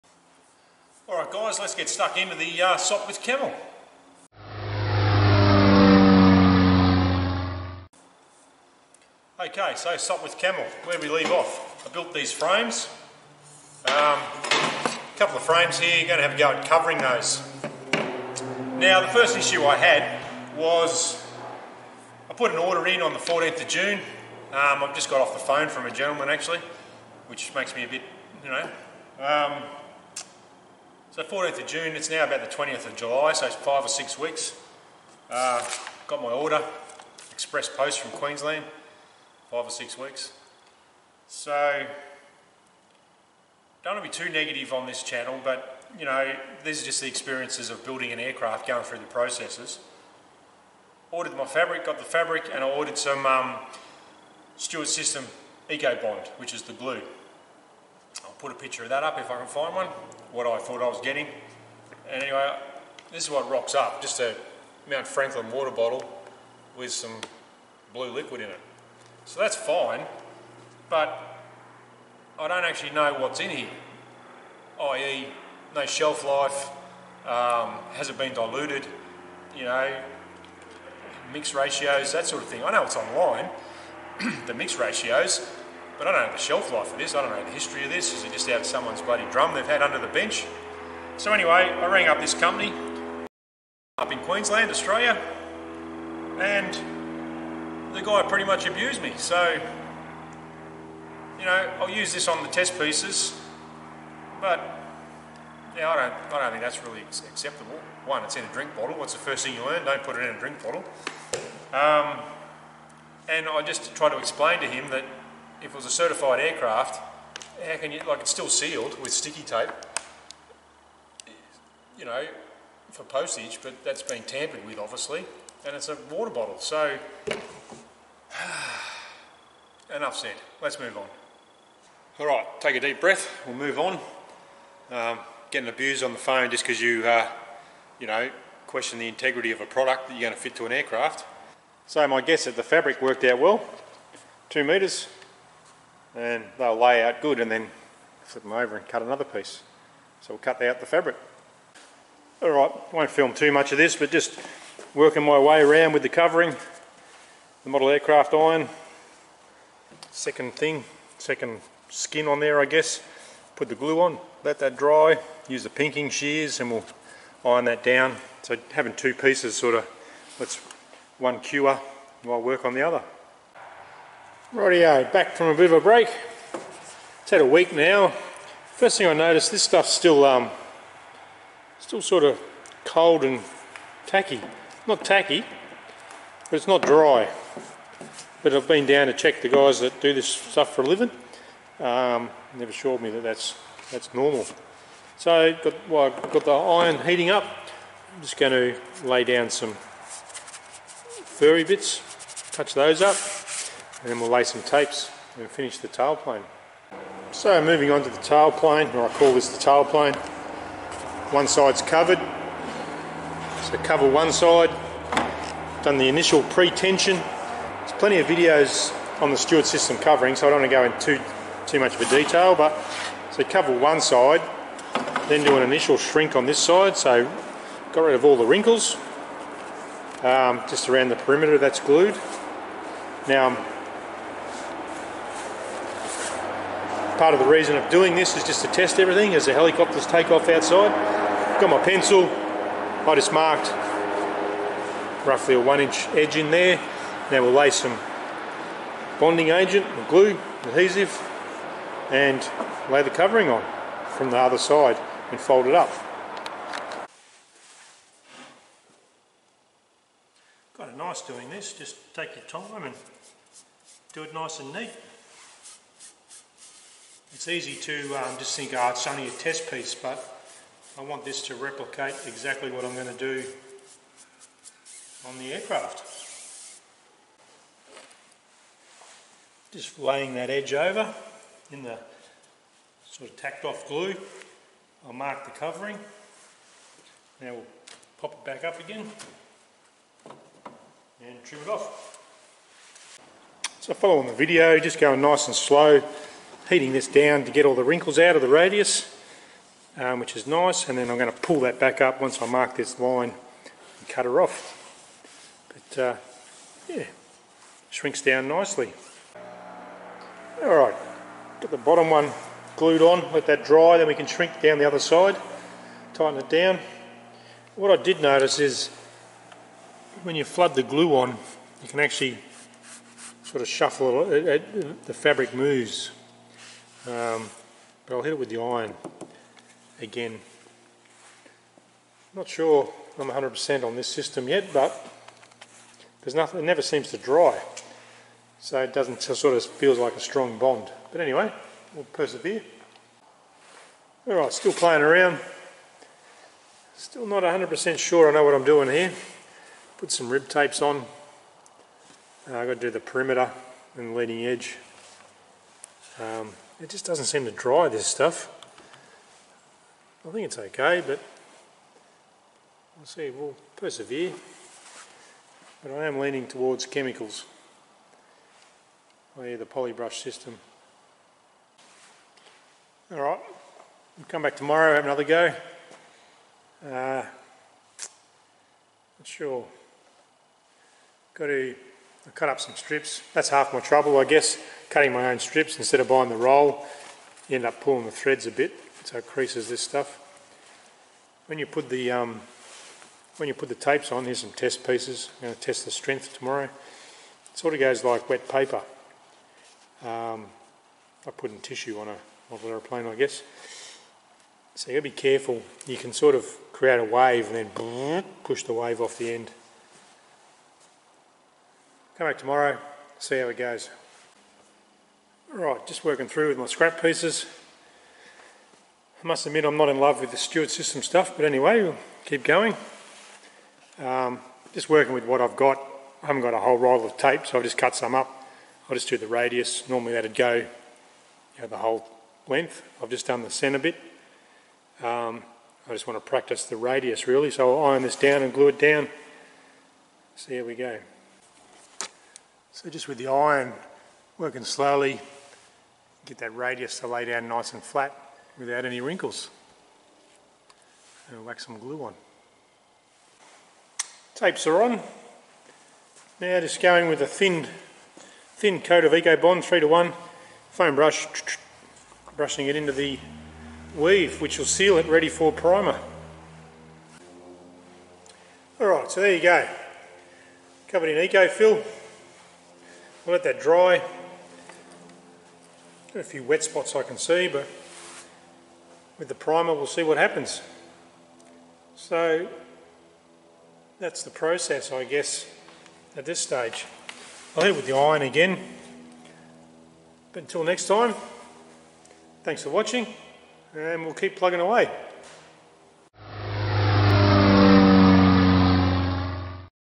Aircraft Covering Stewart System Test
Testing this product for my Sopwith Camel Build.